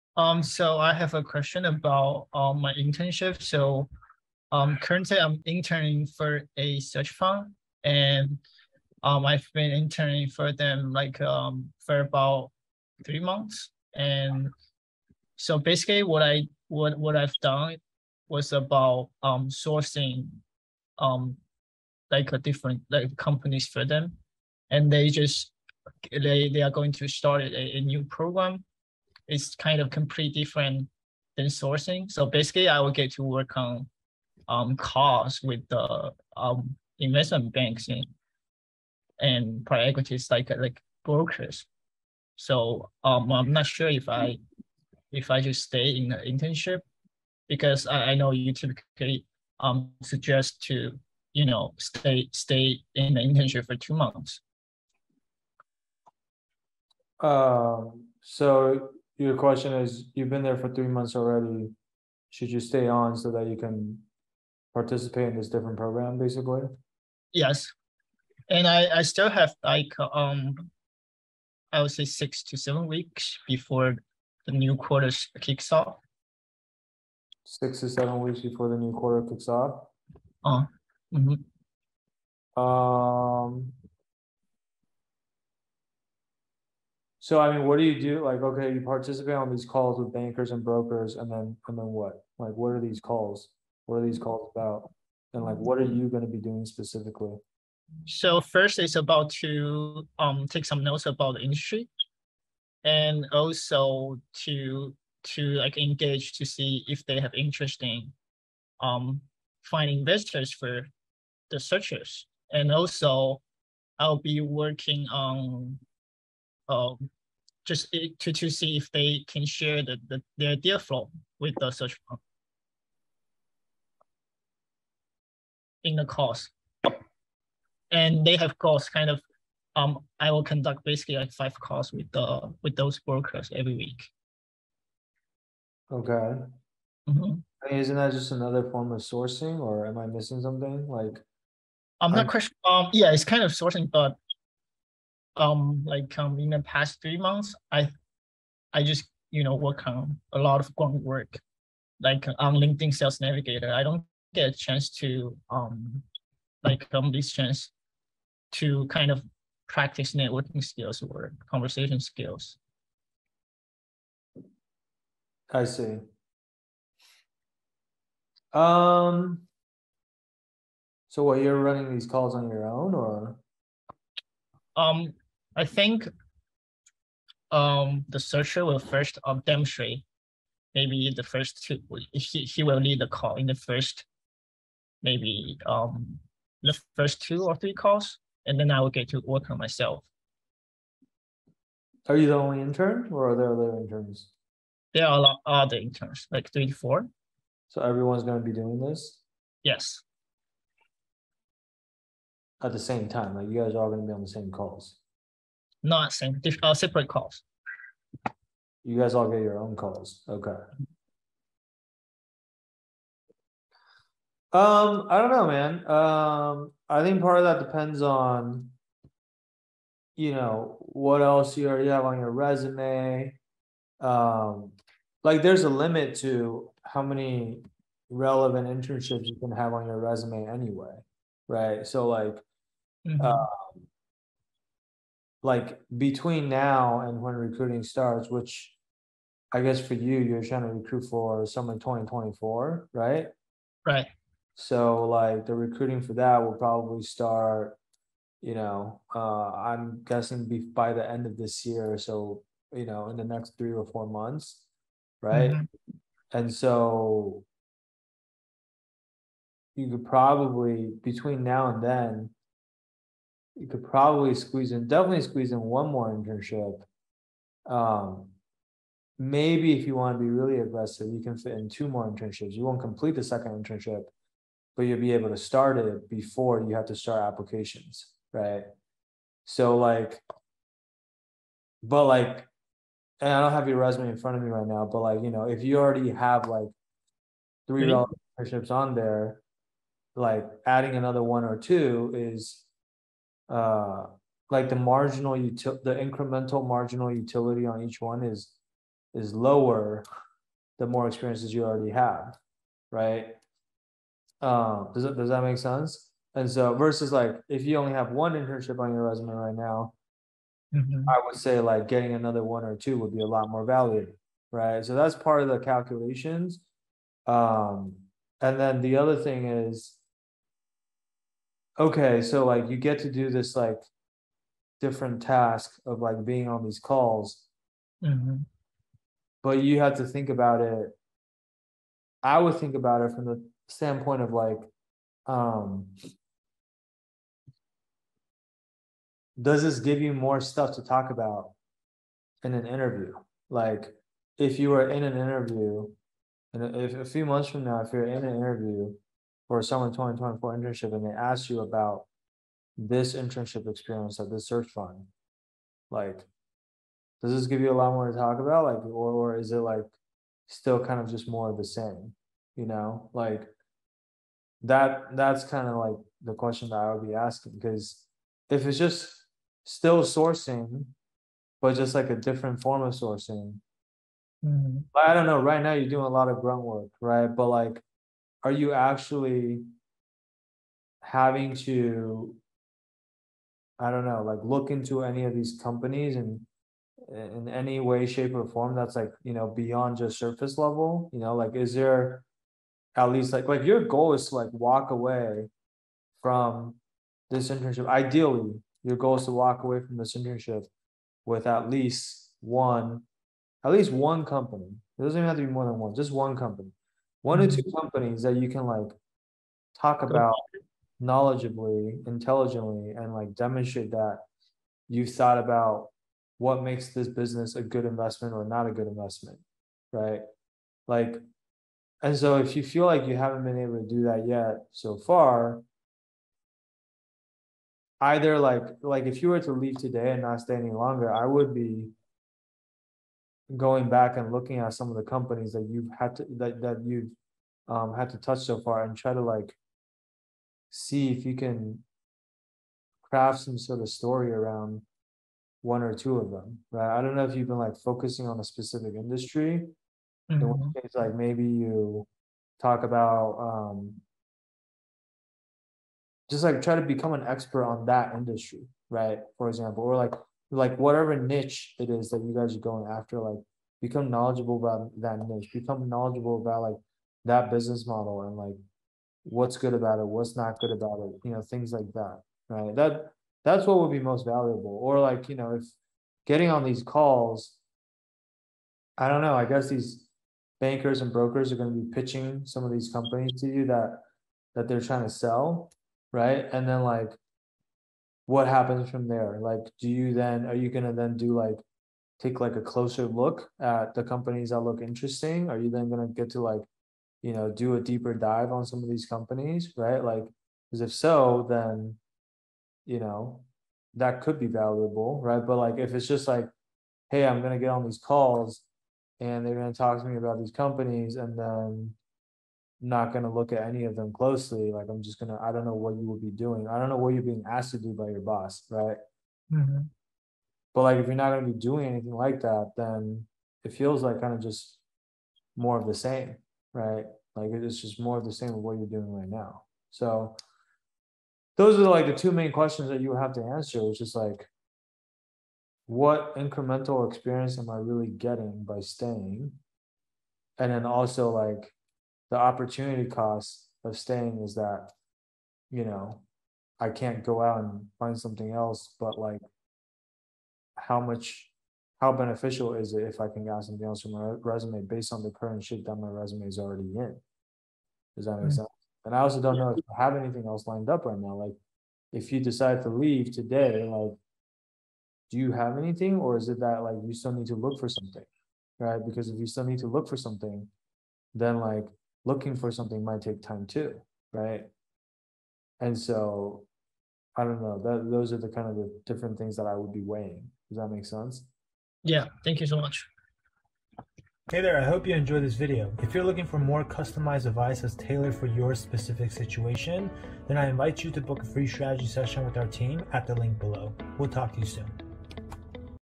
Should You Stay at Your Internship?
A student recently asked me if he should stay longer at his internship, because he’s been promised some new responsibilities if he stays on.

There are a lot of factors that go into this decision, but ultimately it’s a tradeoff between what the incremental learning you’ll get by staying, versus the opportunity cost of being able to add a new internship experience onto your resume instead.

Plus - depending on what other experiences you already have on your resume, that would make a difference on how you make this decision as well.

Watch me walk this student through this decision. Building your resume effectively requires you to allocate your time towards things that will really move the needle, so make sure you learn how to do this.

Book a Virtual Coffee Chat with an Upperclassmen Who Secured an Offer